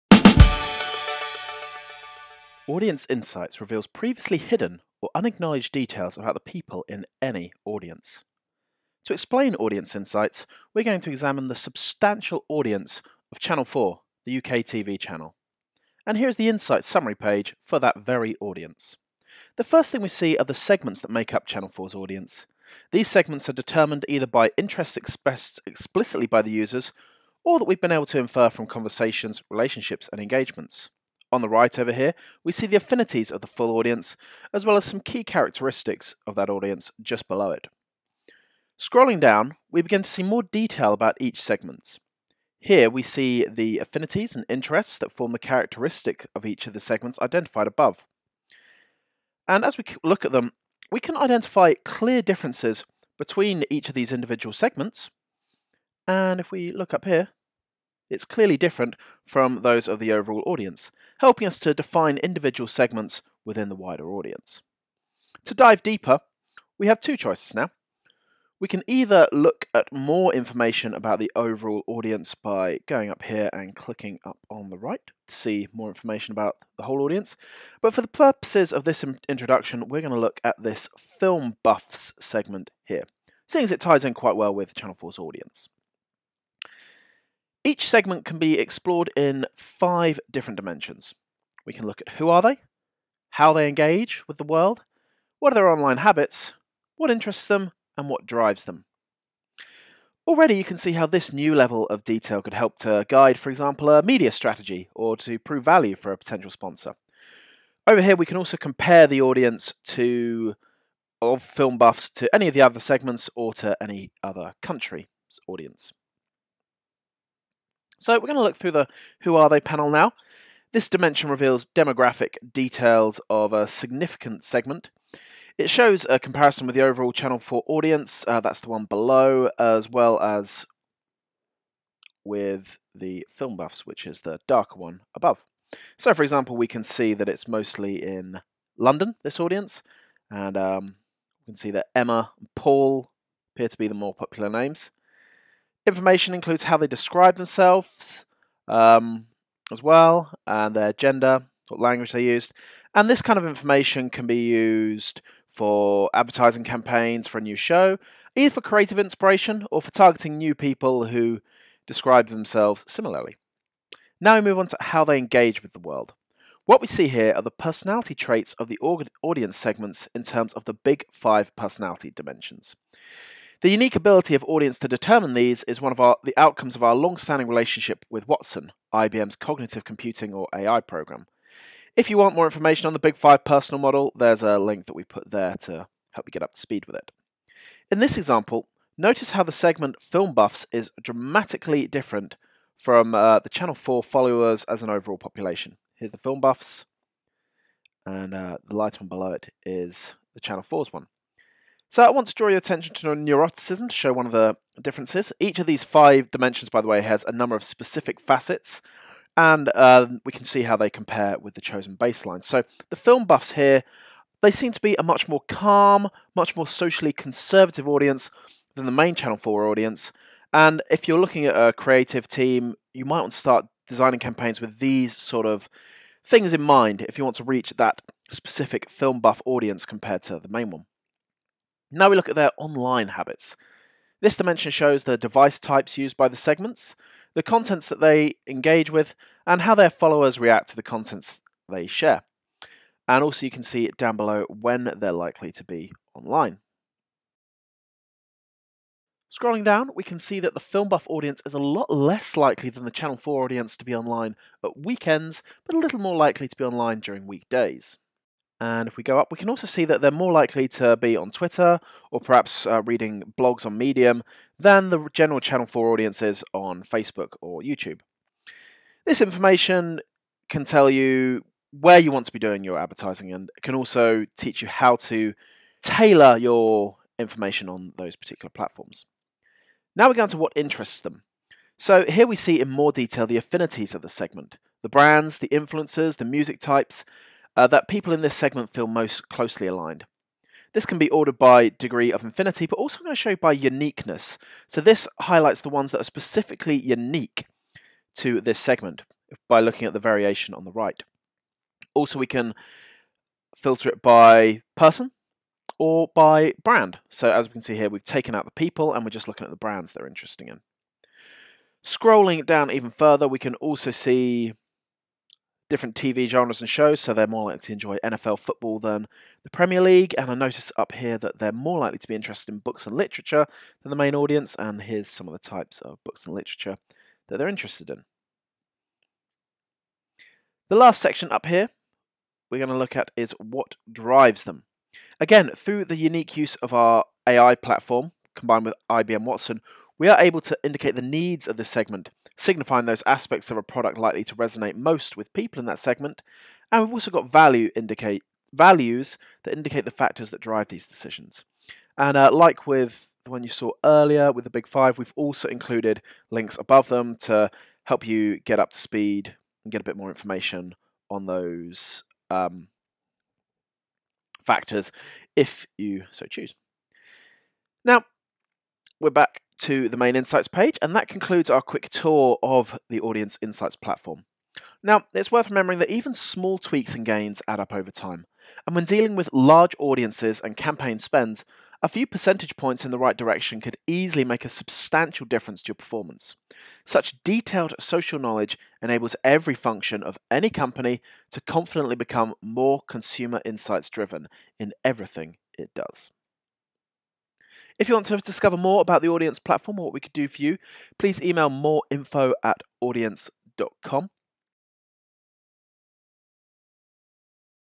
Introducing Audiense Insights
In this video, we give you a quick tour of what Audiense Insights can do.